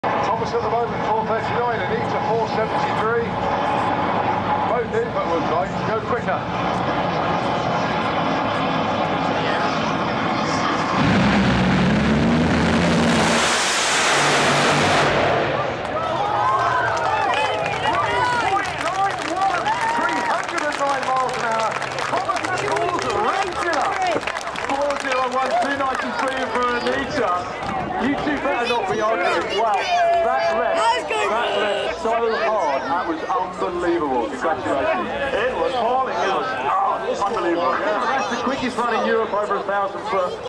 Thomas Nataas runs 3.91 Quickest Top Fuel E.T in Europe - Santa Pod Round 1 2014
Q2 of the first FIA round and Thomas smashes a new low E.T time, breaking the 3-second barrier over the 1000ft top fuel distance for the first time at Santa Pod. It's also the quickest E.T in Europe!